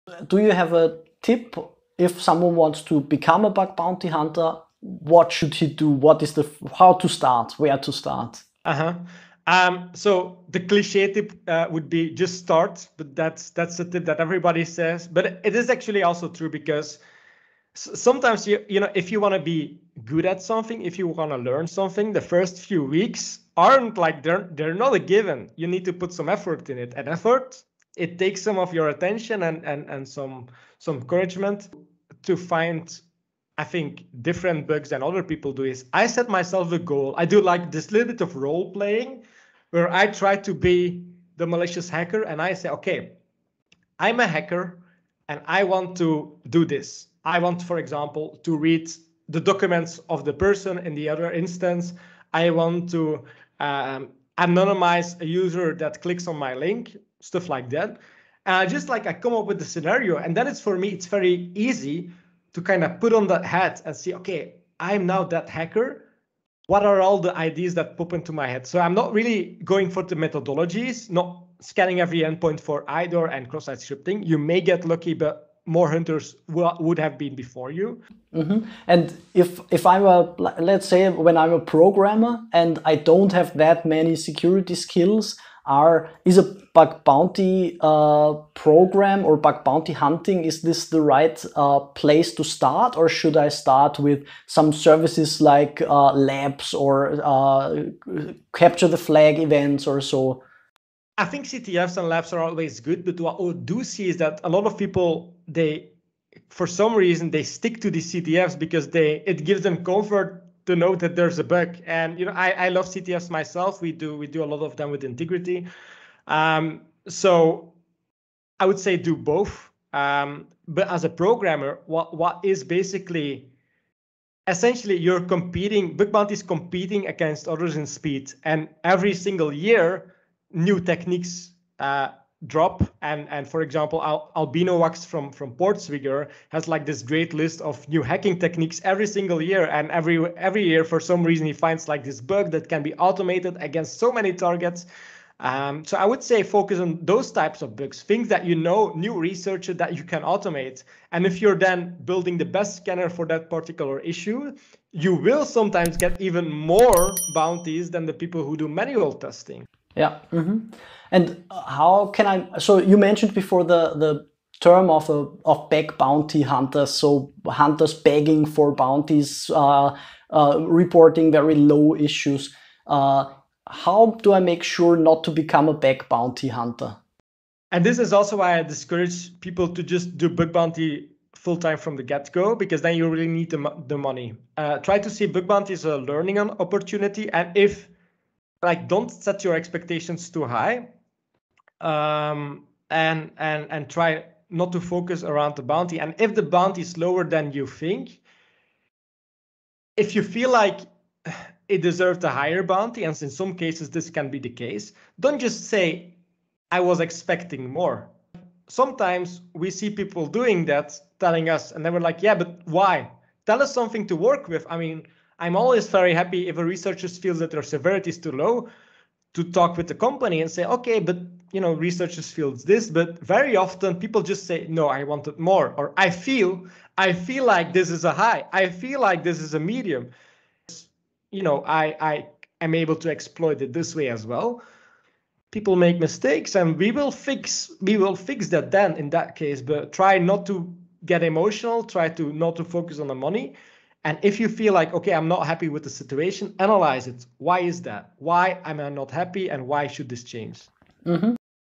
How to start Bug Bounty Hunting // Inti De Ceukelaire from Intigriti on becoming a bug bounty hunter
In this video I asked Inti De Ceukelaire from Intigriti how to take the first steps of bug bounty hunting.
We also talked about Labs and CTFs and how to not become a "beg bounty" hunter.